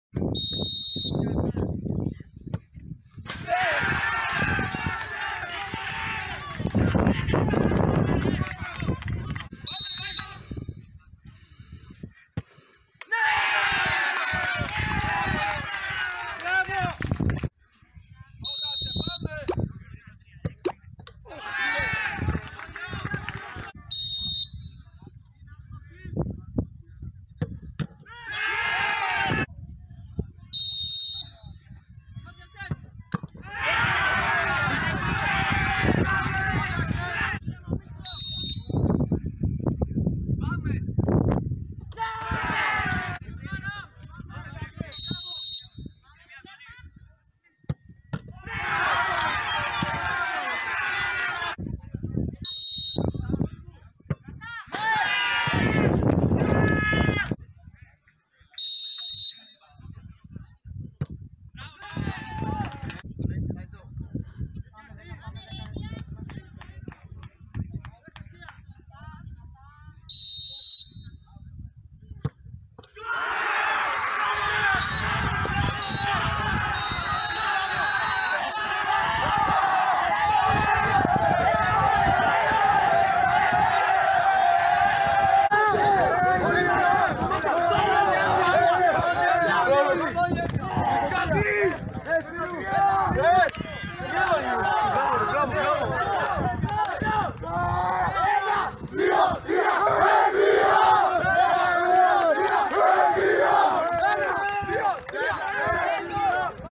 ΜΙΚΤΕΣ ΕΠΣΕ (Κ15): Εύβοια-Ήπειρος 3-2 (πέν., κ.δ.: 0-0) (11/5/2022)
Η διαδικασία των πέναλτι στον αγώνα Εύβοια-Ήπειρος, για τη δεύτερη φάση του πανελλήνιου πρωταθλήματος Κ15 προεπιλογής εθνικών ομάδων.